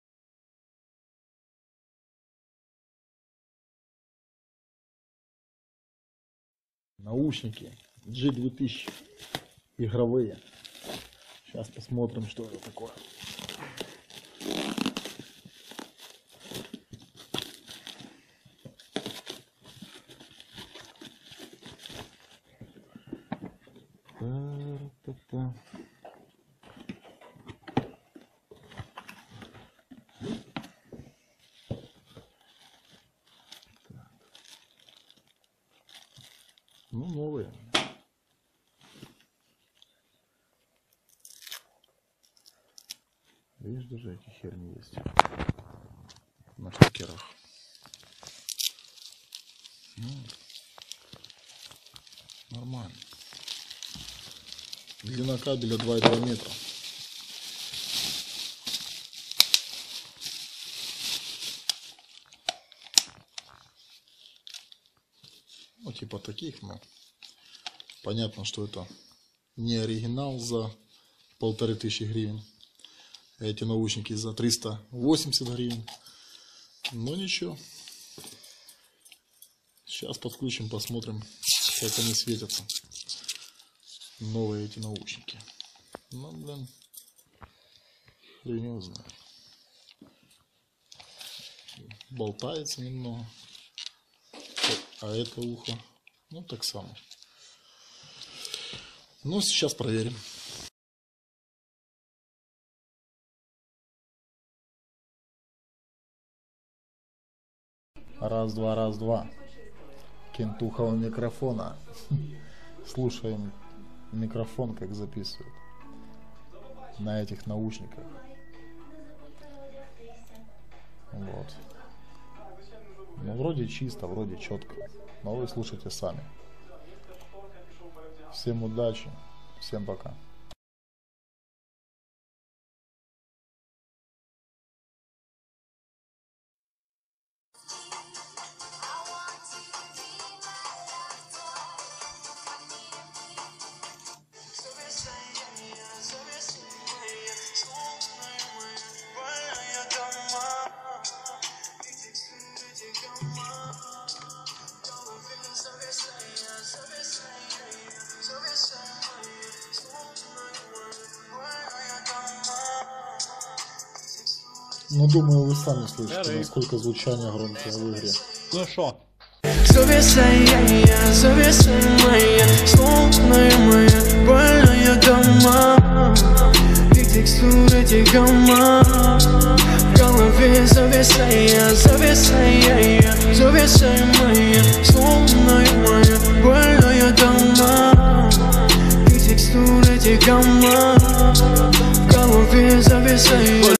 ОБЗОР НАУШНИКОВ G2000
Игровые наушники G2000 РЕКОМЕНДУЮ. СЛЕДУЮЩЕЕ ВИДЕО ПРО СИСТЕМНЫЙ БЛОК(КОРПУС VINGA). НЕ ПРОПУСТИ!!!